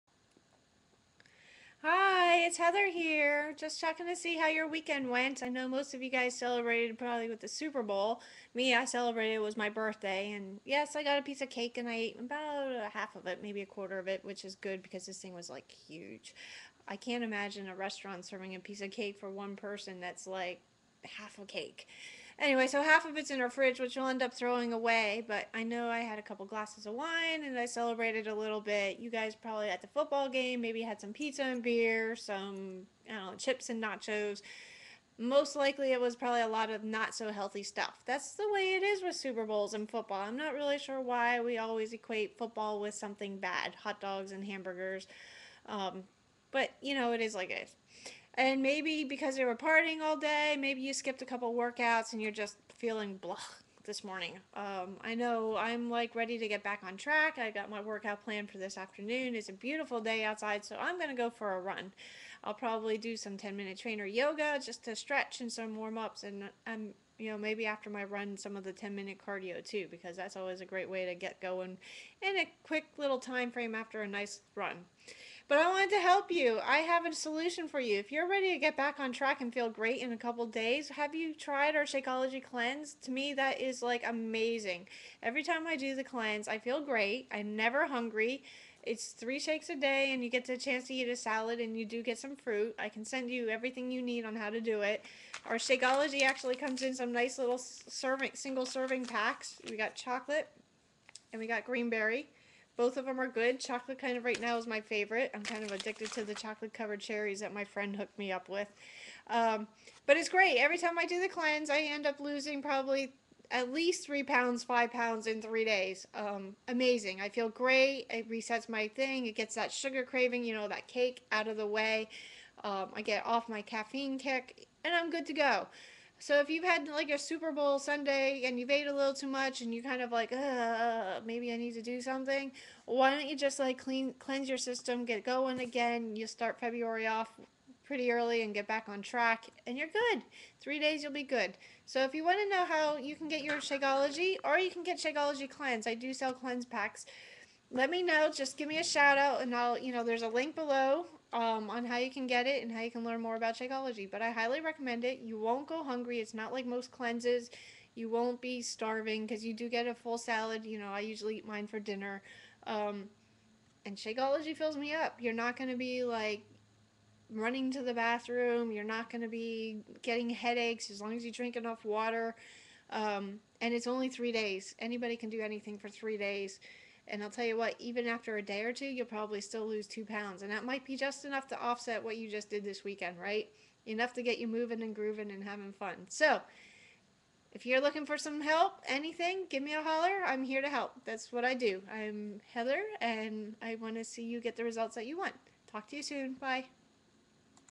Get Back on Track after too much Super Bowl Partying!
Get back on track after a weekend of overdoing it with the Super Bowl!  Try our 3 day Shakeology Cleanse and feel amazing and on track!  Link above takes you to my Shake Cleanse Packs!  Why not give it a try!  I know I will!